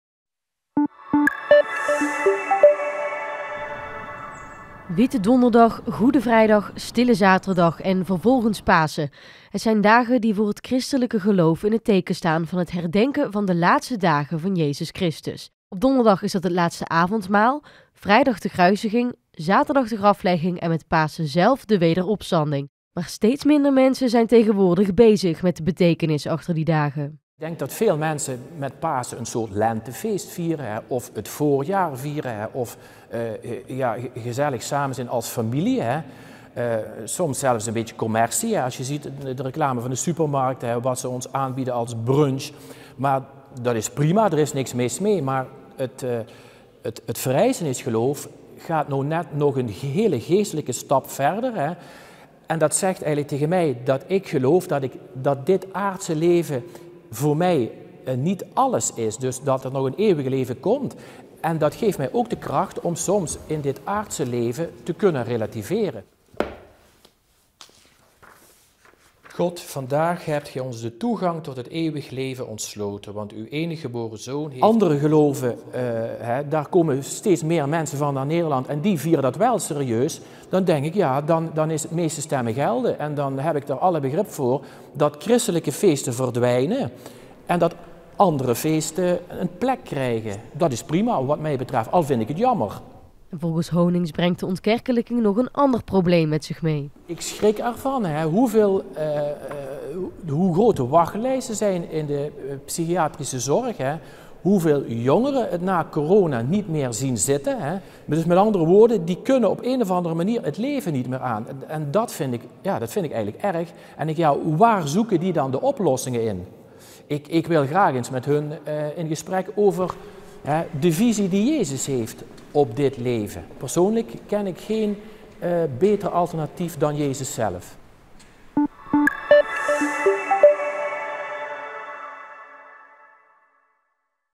Ontkerkelijking met Pasen: 'Meer een lentefeest' | ZO-NWS
Steeds minder mensen weten wat de Goede Week, het herdenken van de laatste dagen van Jezus, precies inhoudt. Op Witte Donderdag herdenken christenen het Laatste Avondmaal, Goede Vrijdag staat in het teken van de kruisiging van Jezus, op Stille Zaterdag staan mensen stil bij de graflegging en met Pasen zelf de wederopstanding.

'Maar het is nu meer een soort lentefeest", vertelt deken van Schinnen, Jack Honings.